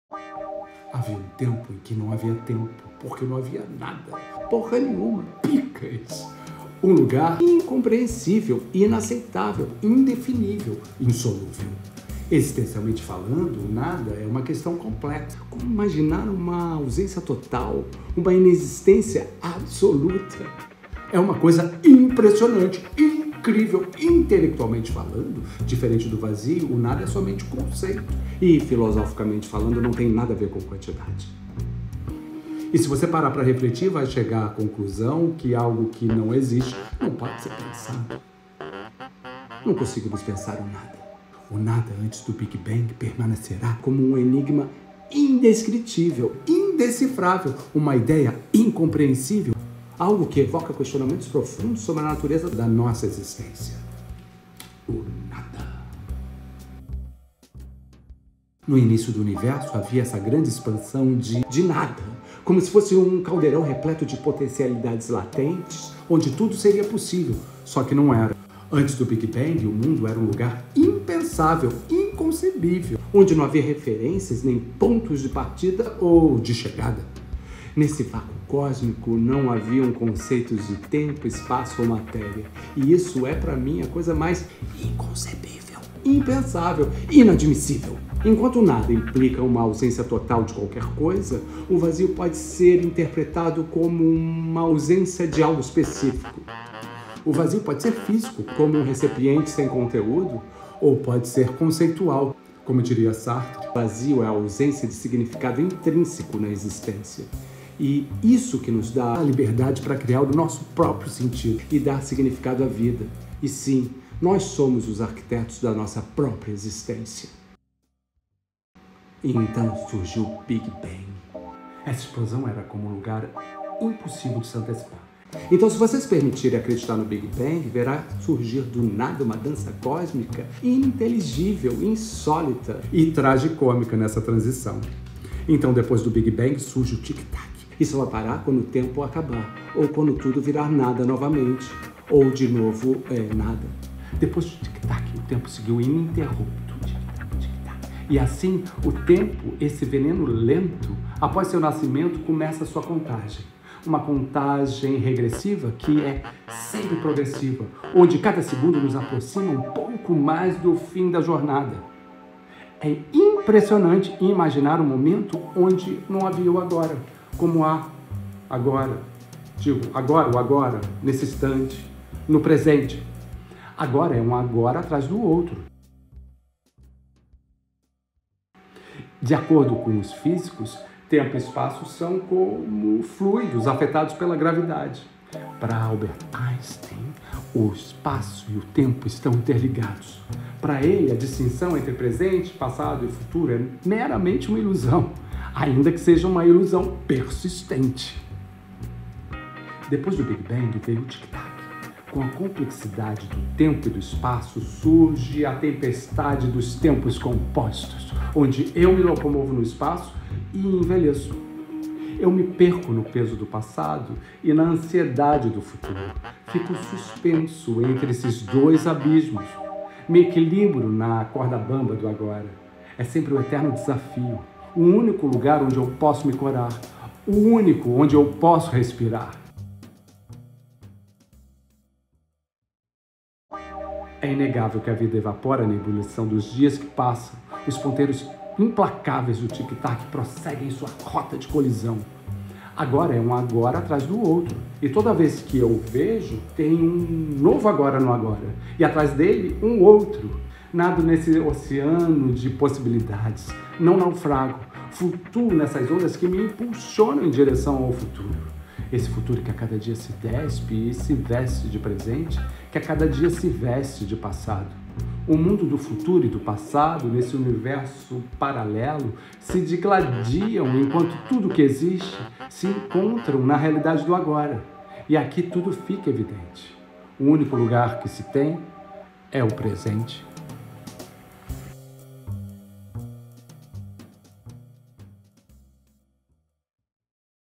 O Lugar Inaceitável, Improvável, Insolúvel.O vazio de Sartre.
Antes de haver o tempo, existia o nada. O conceito é assombroso e suscita mais perguntas do que respostas. Ao abordar este tema inicia-se uma incrível exploração do vazio primordial. Adentrar neste tema é trilhar uma jornada única e fascinante! É um mergulho nas profundezas do desconhecido. AntesDoTempo Nada Mistério JornadaÚnica Fascinante ConceitoTempo Mistério ONada Inaceitável Improvável Insolúvel"